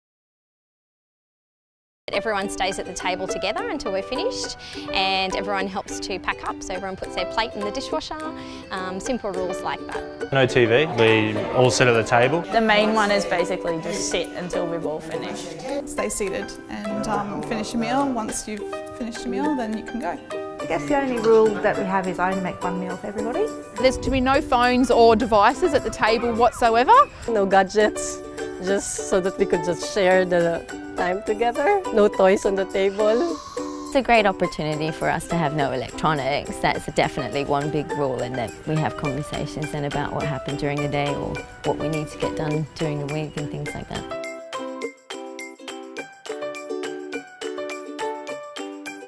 Dinner Time Rules - Talking with families
How hard is it to sit still and eat a meal? Apparently it’s more difficult than us adults seem to understand. We spoke to parents who said staying seated was one of the most common rules for meal times. Do you have any more suggestions for making meals as enjoyable as possible, for everyone?